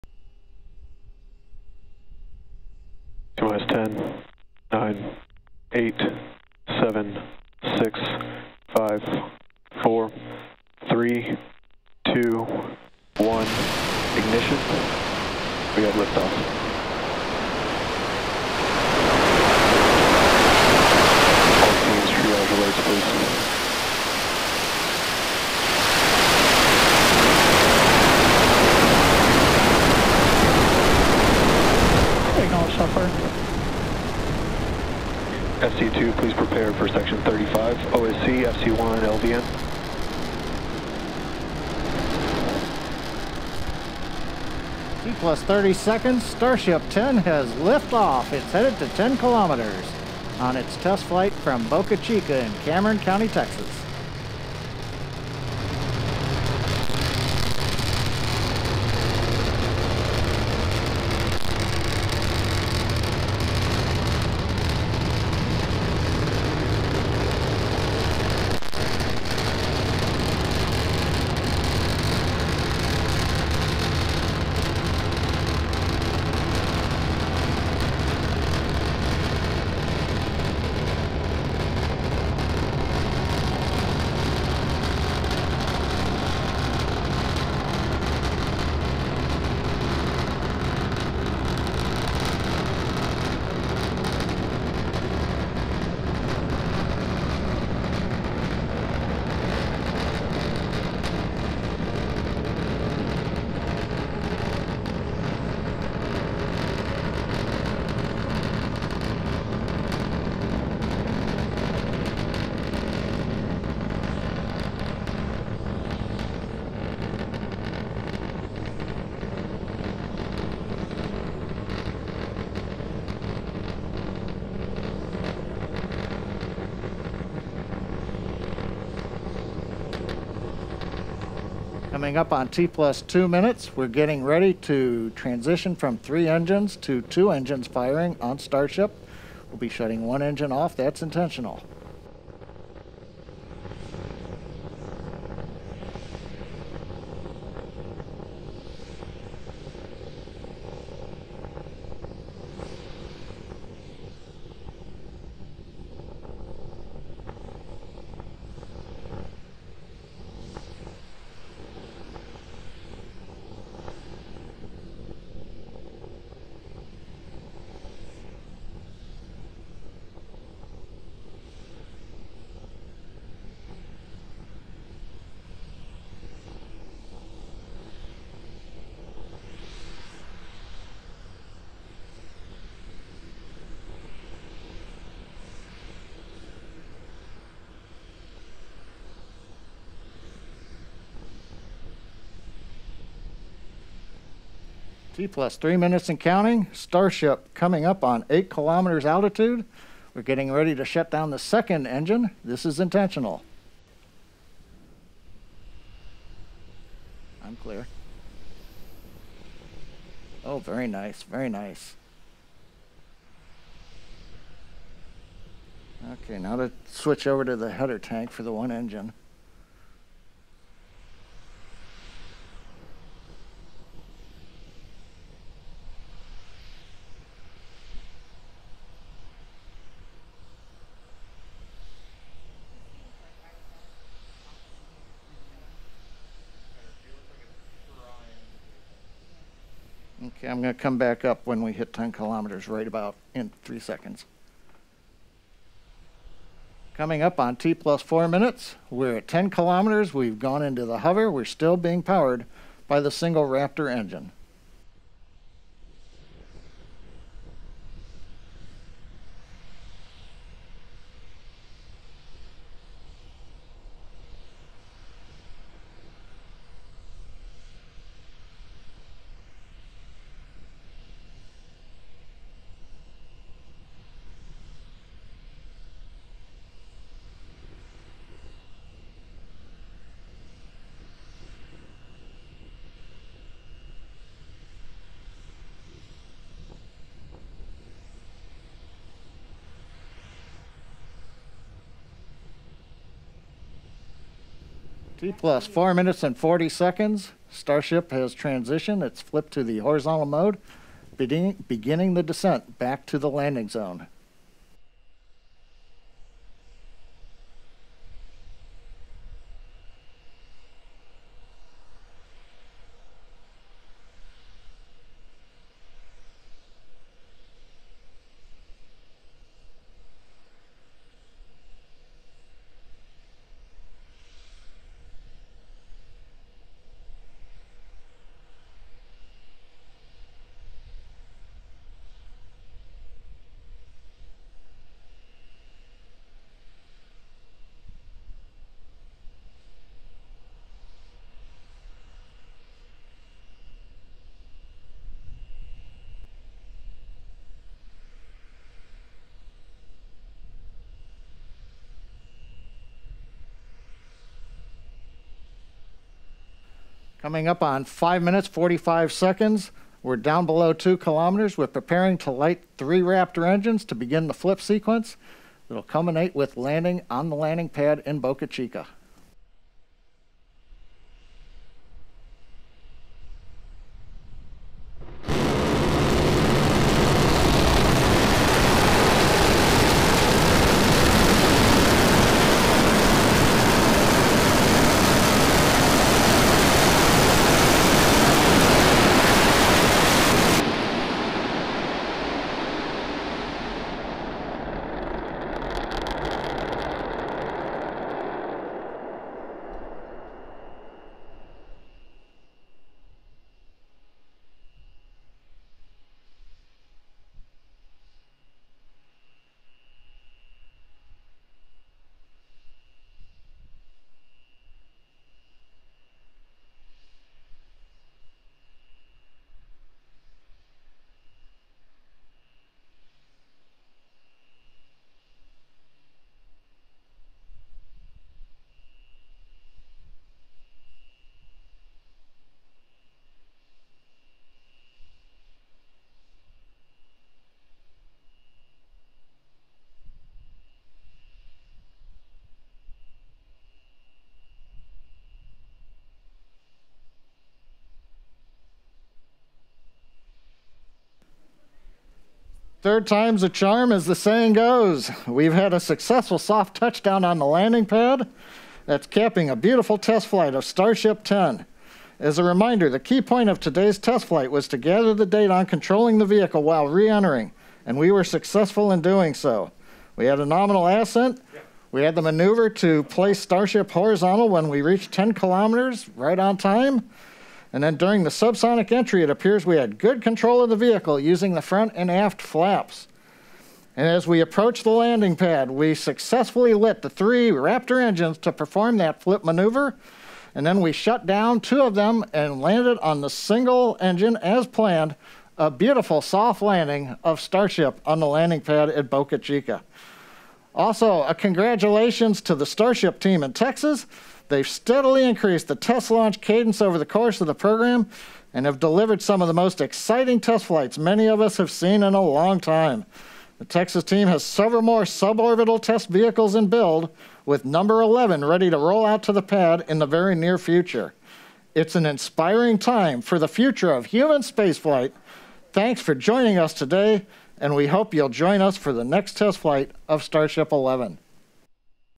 SpaceX Starship SN10 soars, lands for first time!
SpaceX Starship SN10 prototype flew to an altitude of 6.2 miles (10 kilometers) and landed 6 minutes and 20 seconds after liftoff on Mar. 3, 2021. Unfortunately, it exploded several minutes after touchdown - see it

Credit: SpaceX